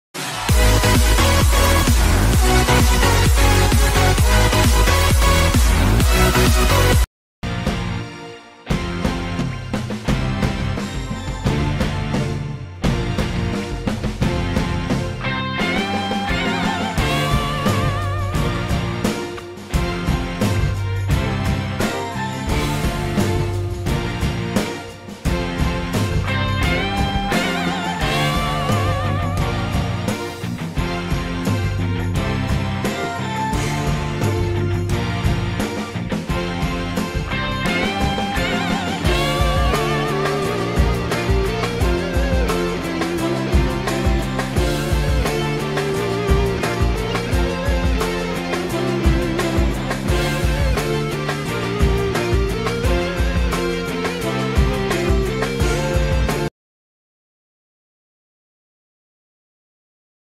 Amazing Performance in Nanchaku by Commando Haroon's Student#Haroon#martial#arts
کمانڈو ہارون کے شاگرد نے نن چکو میں کمال کر دیا
Pakistan Mix Martial Arts Club